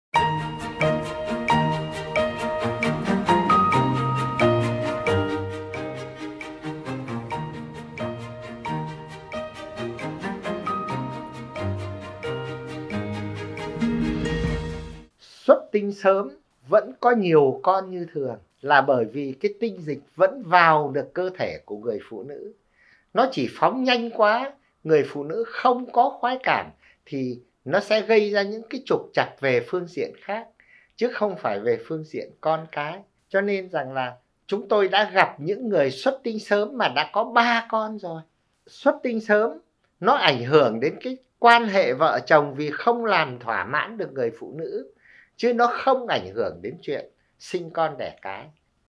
Xuất tinh sớm có phải là nguyên nhân gây vô sinh không ?
Vợ chồng em hơn 1 năm rồi chưa có em bé, em thường bị xuất tinh sớm, có khi mới đưa vào đã xuất tinh, không biết điều này có gây cản trở khả năng thụ thai hay không vì em vẫn cho tinh trùng vào trong vợ, nhưng đến nay vẫn chưa thấy gì ạ!
Mời bạn đọc xem phần tư vấn của NGND.GS.TS.BS Trần Quán Anh - Trưởng khoa Tiết niệu Nam học - Bệnh viện Đa Khoa Tâm Anh, Hà Nội về vấn đề này.